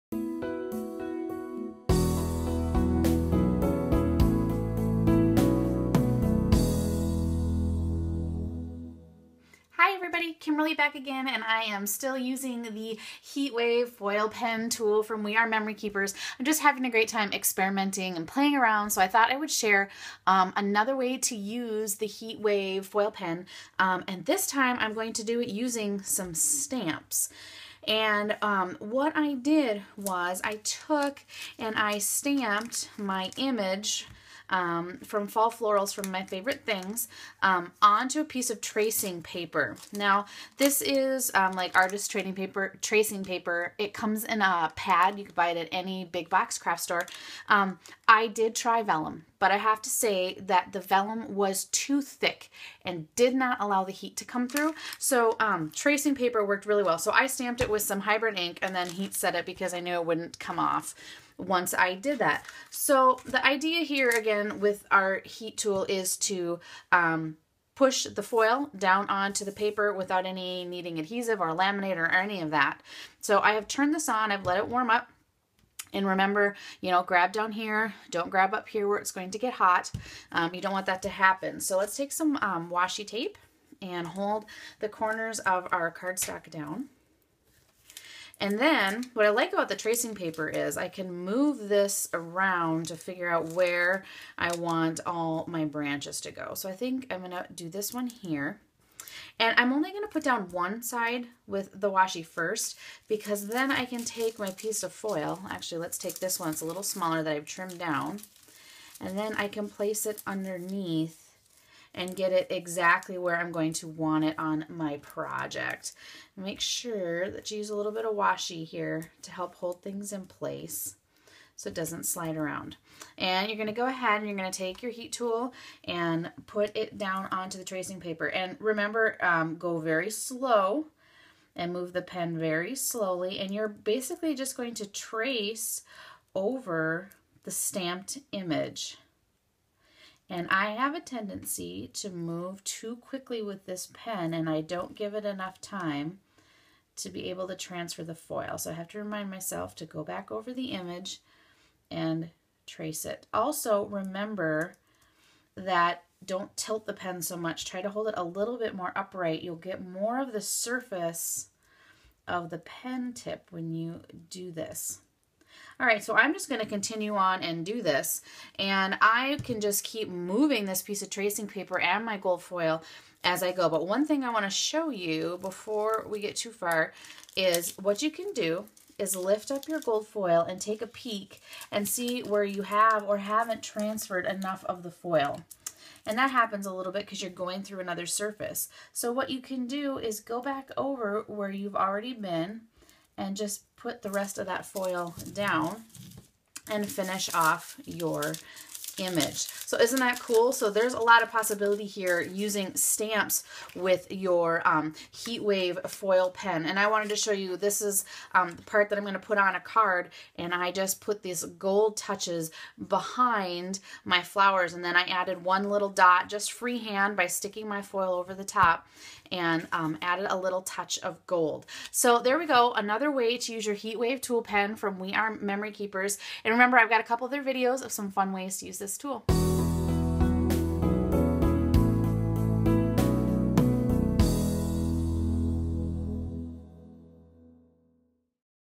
WRMK foil pen with stamps
In this video, Kimberly Crawford will show you how to use your WR Memory Keepers Heat Wave Foil Pen with stamps! See how to use tracing paper to add foil accents to your projects with your Foil pen.